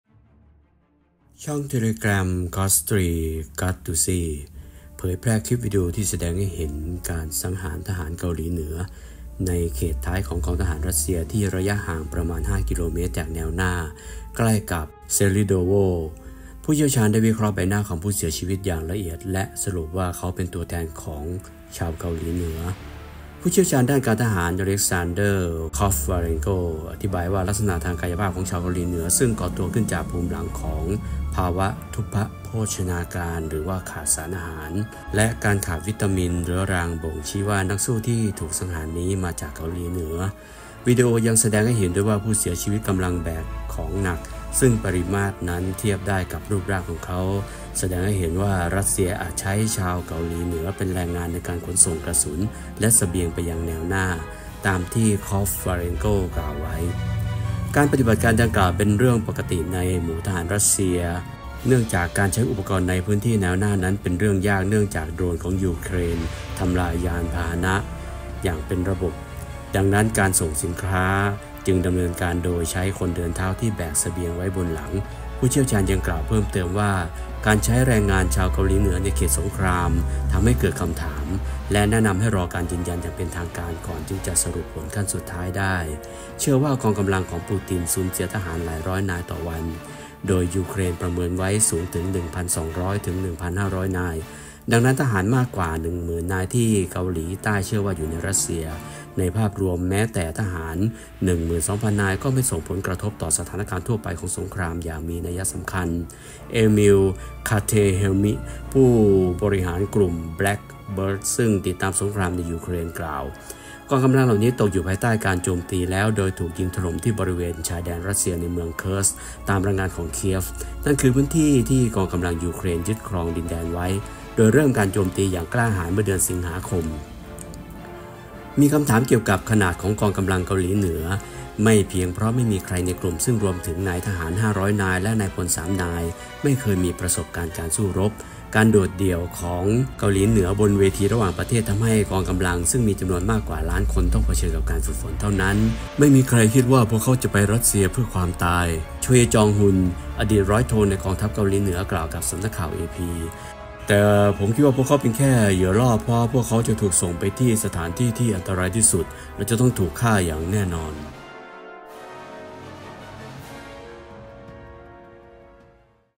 กองทัพยูเครนเผยคลิปวิดีโอให้เห็นว่าทหารเกาหลีเหนือไปทำอะไรที่แนวรบเซลิโดโวกันแน่
คาดไว้ไม่ผิด ปูตินใช้ทหารของประธานคิมเป็นกองทัพมดไว้ขนเสบียงนี่เอง